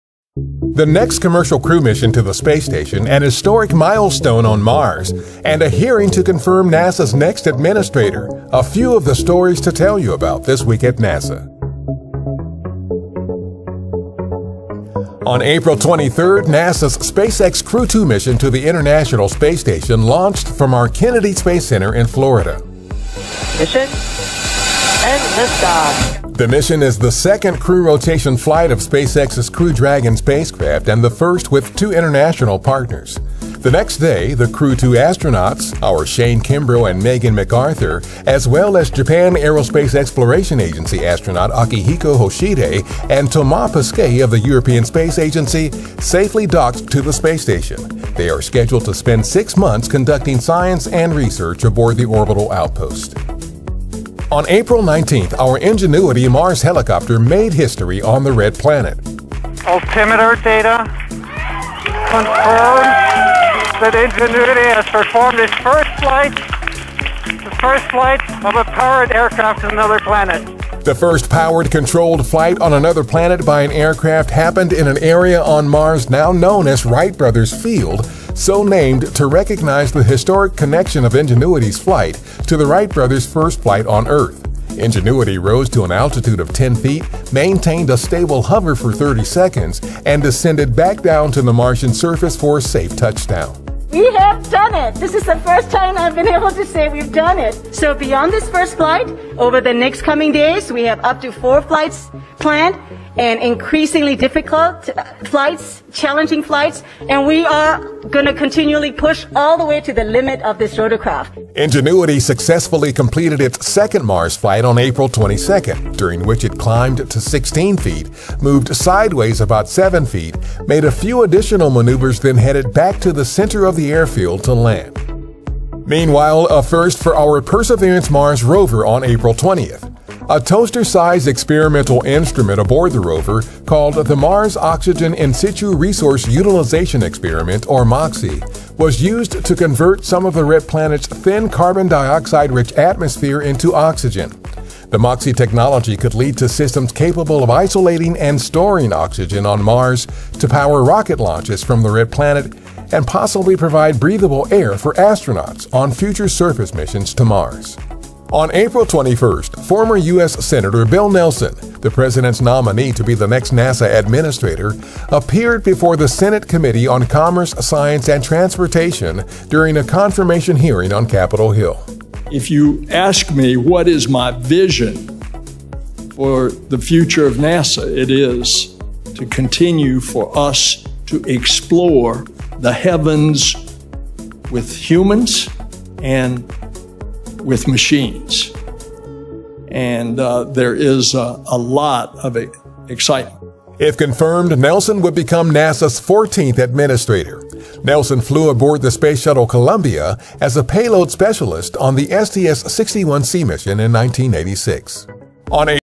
Historic Milestone on Mars + Commercial Crew mission to Space Station on this week @ B and M Tech
This is a historic milestone to Mars. The next commercial crew mission to the space station, a historic milestone on Mars, and a hearing to confirm NASA’s next administrator … a few of the stories to tell you about – This Week at NASA!